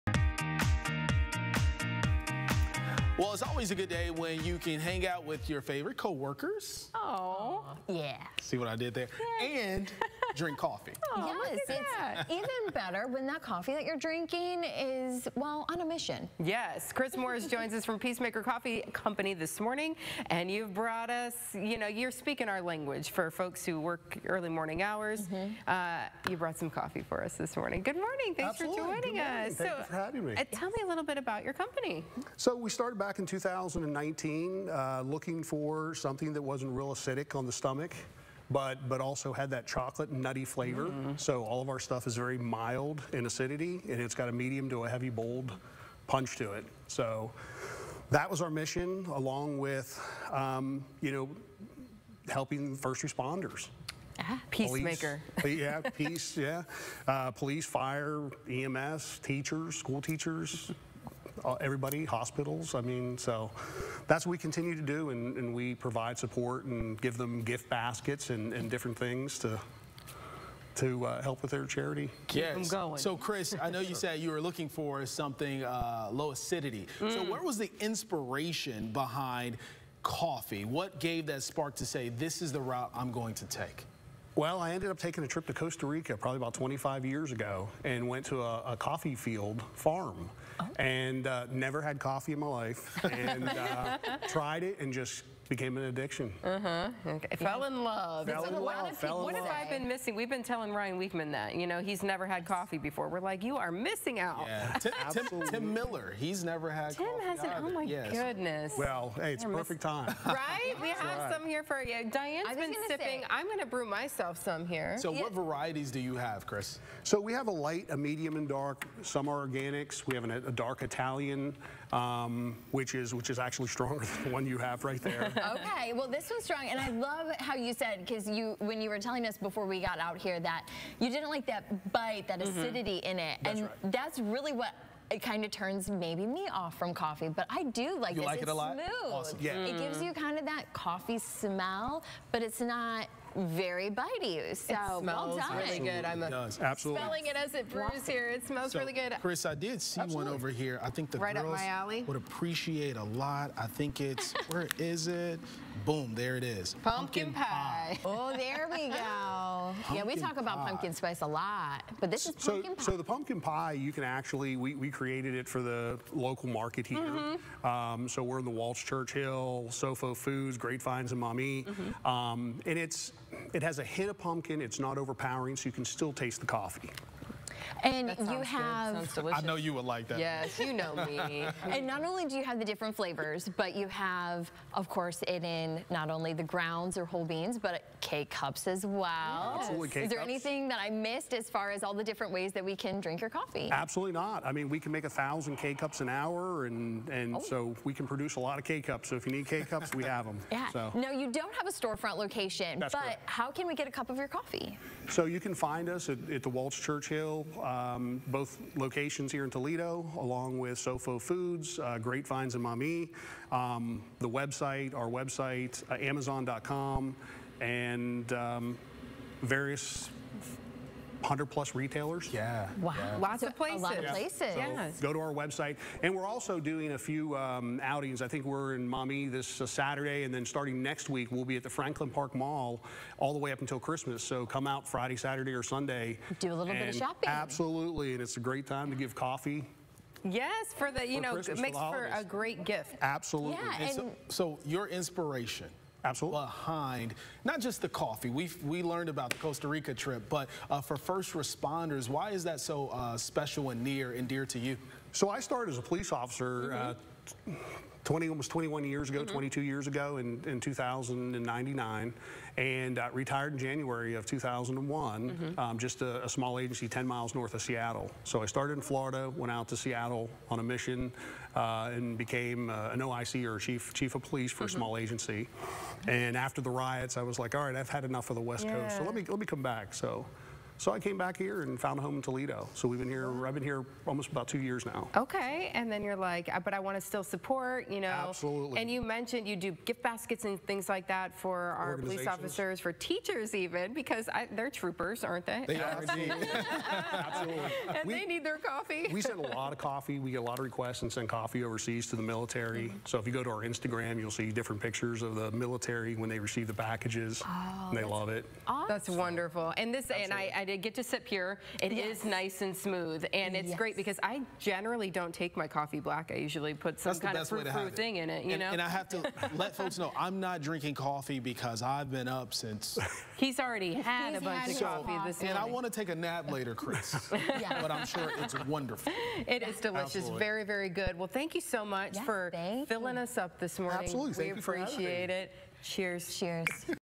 Local coffee roaster proceeds support first responders, service members | Good Day on WTOL 11
Peacemaker Coffee, a local coffee roaster, supports veterans and first responders. It is available for purchase at many northwest Ohio retailers.